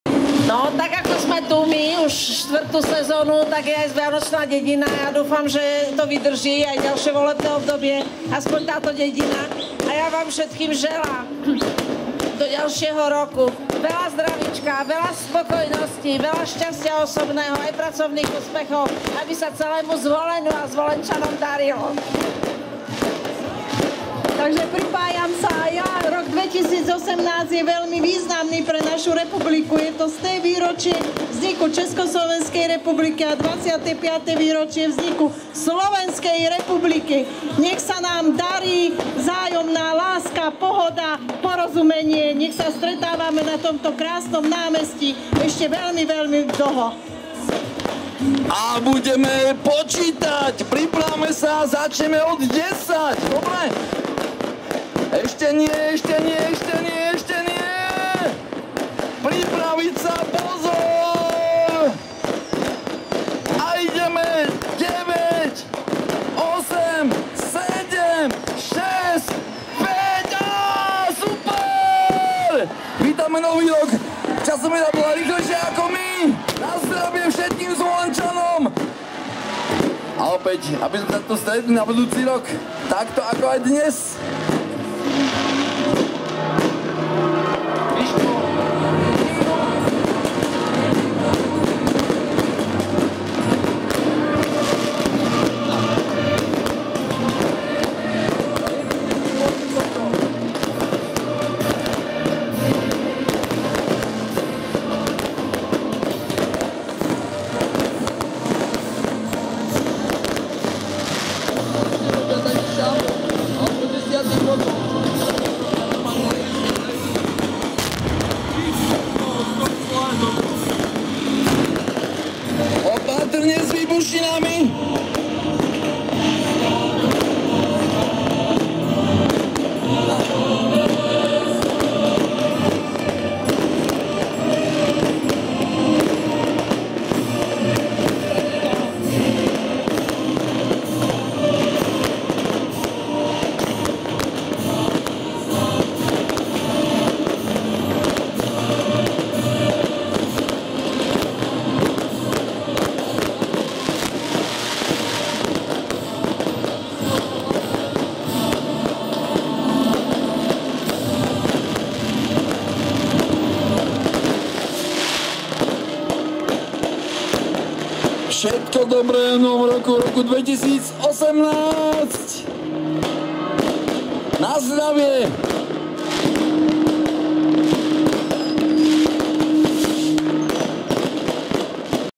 Oslava Silvestra 2017 Zvolen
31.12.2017 po hudobnom vystúpený kapely Creedence revival – tribute band došlo na pódiu vo Vianočnej dedine na obligátny prípitok a želania.

Tentokrát opäť bolo možné na pódiu vidoeť po boku primátorky mesta Zvolen Lenky Balkovičovej aj poslankyňu MsZ Alžbetu Staníkovú.